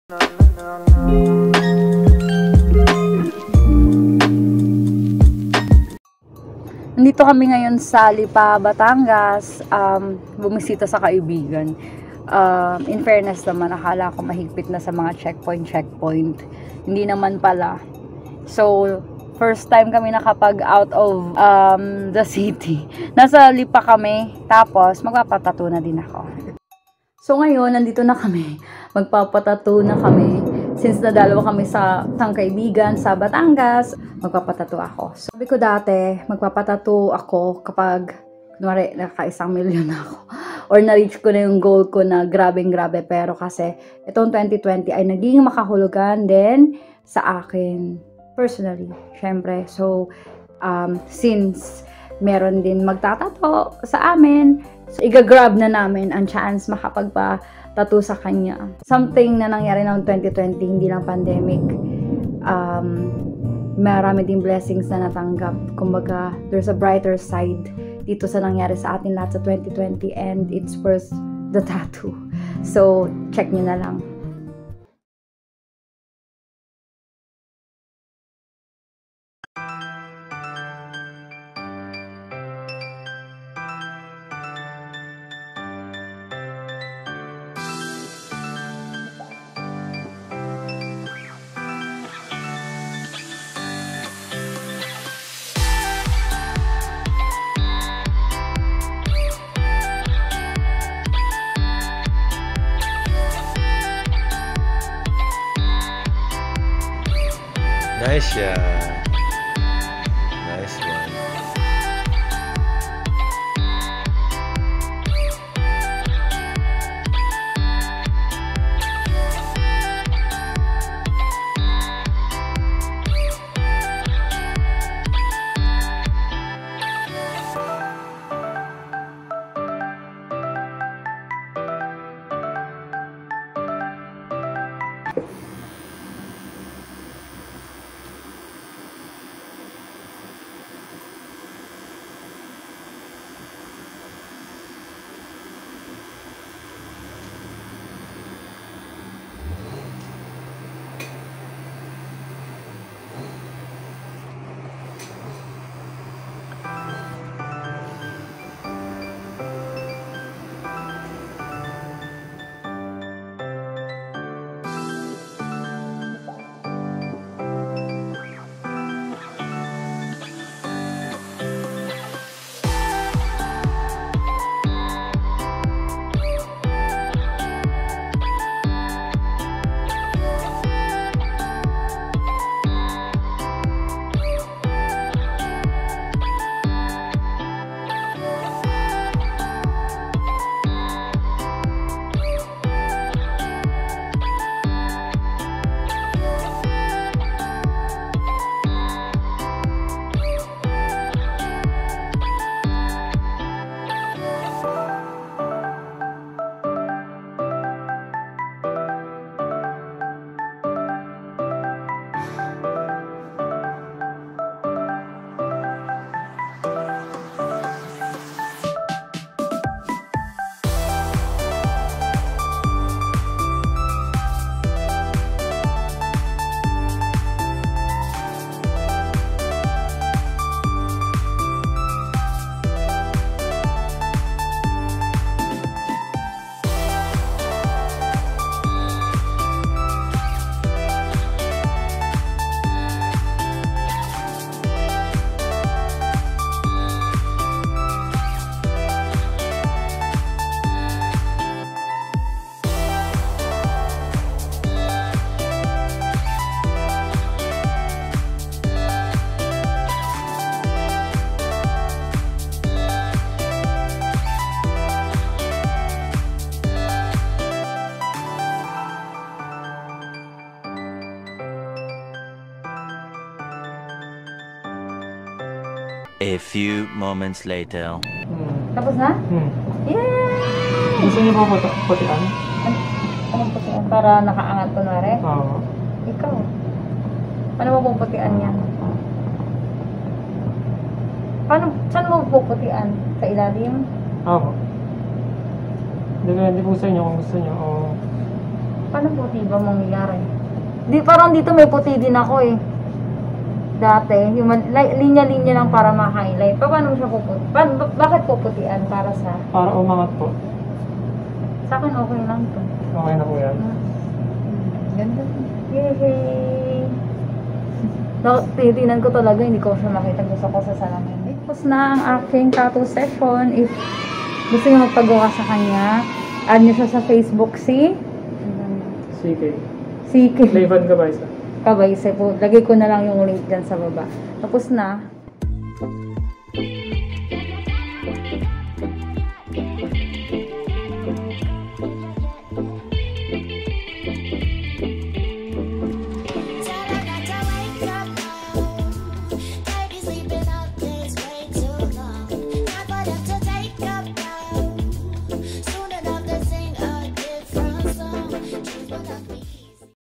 Got Tattooed After The Lockdown!
I got a new tattoo Pandemic-Induced =)
Tattoo Artist: @CK Cabaysa

Note: The place and all equipment were properly sterilized/ sanitized. Face mask was required during the tattoo session.